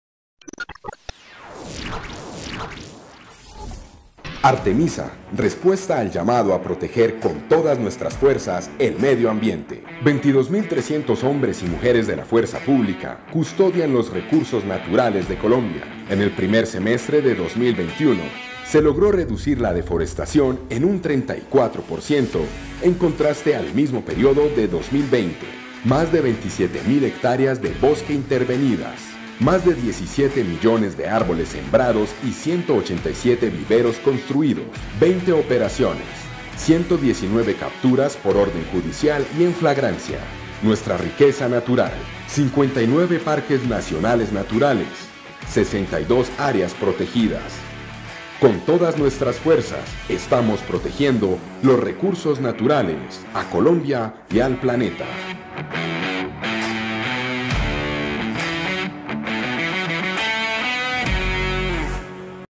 Estrategia Artemisa: protegemos la riqueza natural de Colombia
EstrategiaArtemisa, la Fuerza Pública protege los recursos naturales del país. Con todas nuestras custodiamos nuestro medio ambiente , la repuesta al llamado por la defensa de la biodiversidad y el ecosistema